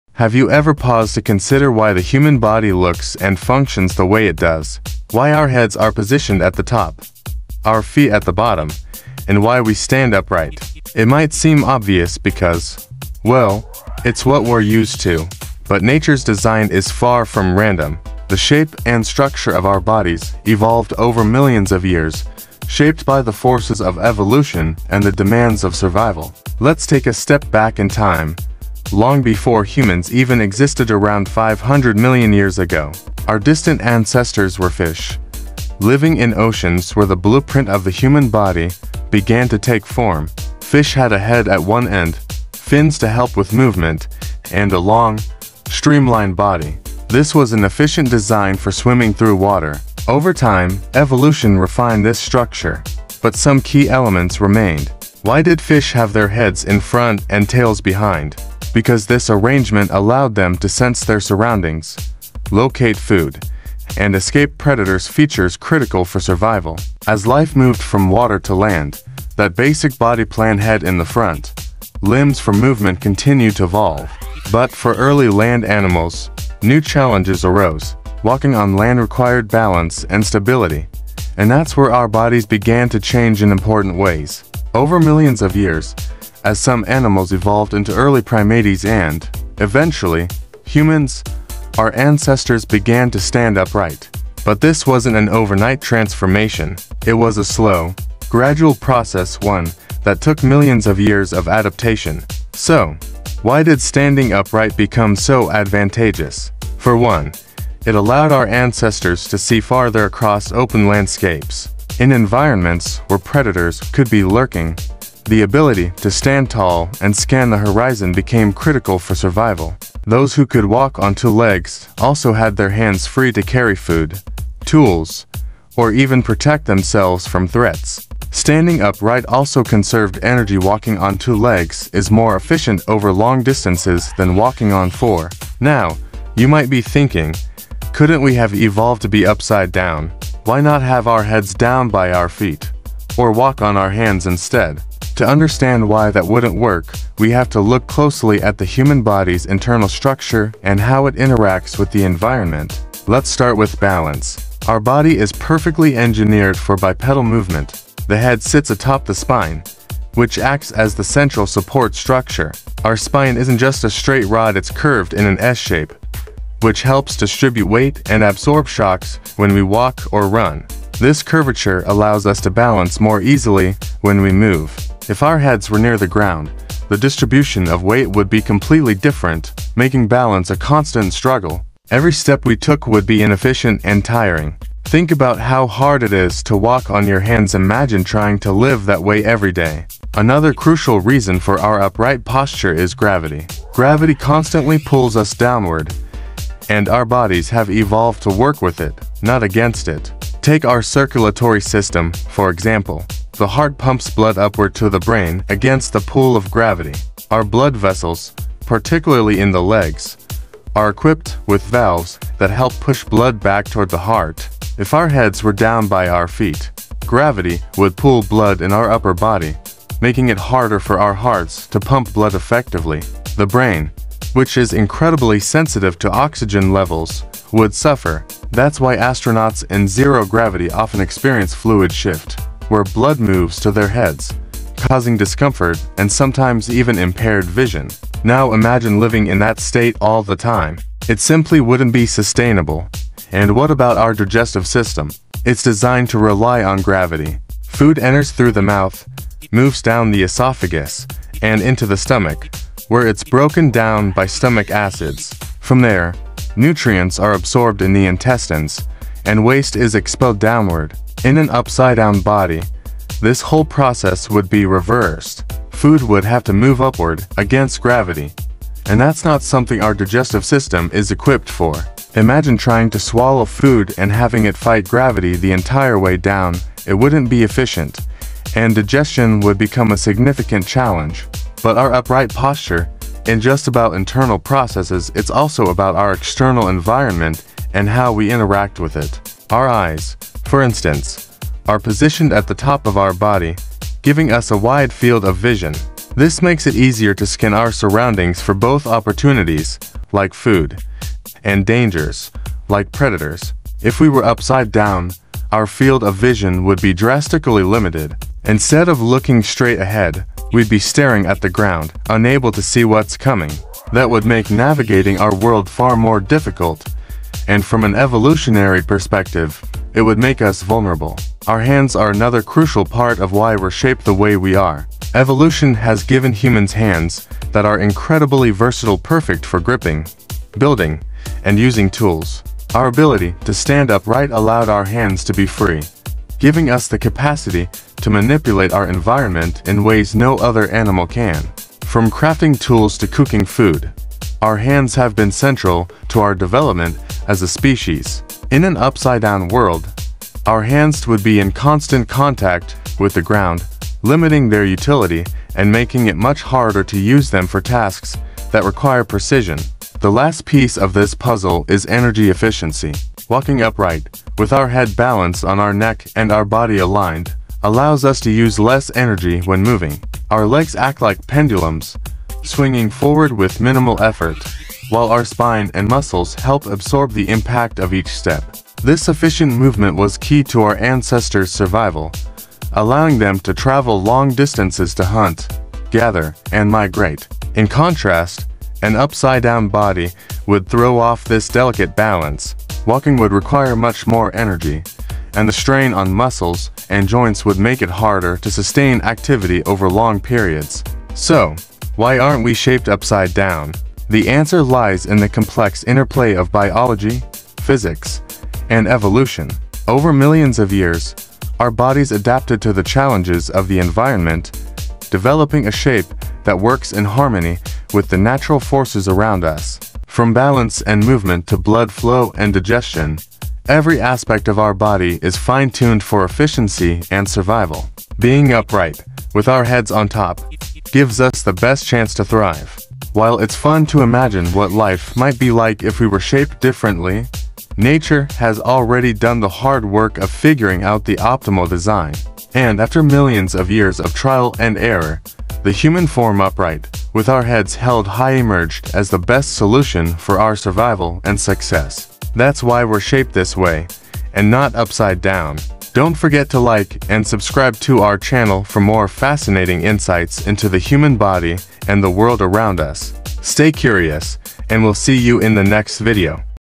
Why Is Our Body Shaped Like This?
In this fascinating video, we explore the evolutionary reasons behind why the human body is shaped the way it is and why we stand upright instead of upside down. Through a journey spanning millions of years, from our early fish ancestors to modern humans, we uncover how balance, gravity, digestion, and survival needs shaped our body’s structure. We also dive into the roles of our circulatory, skeletal, and digestive systems in making upright posture the optimal design for human life. If you've ever wondered why we aren’t walking on our hands or why our heads aren’t near the ground, this video breaks it down in simple terms.